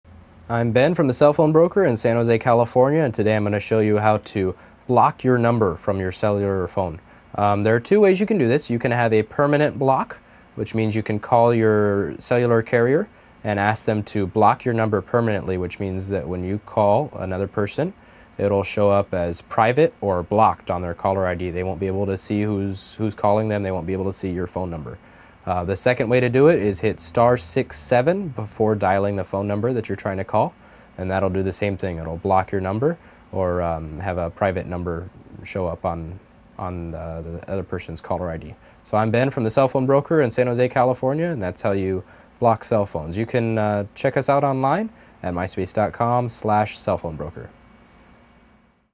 Cell Phone & Cell Phone Providers : How to Block My Number With Sprint
To block a number with Sprint, either call the carrier to permanently block the number or dial "*67" before making a call to conceal your number from the other person's phone. Get a cell phone number blocked with information from a wireless specialist in this free video on cell phones.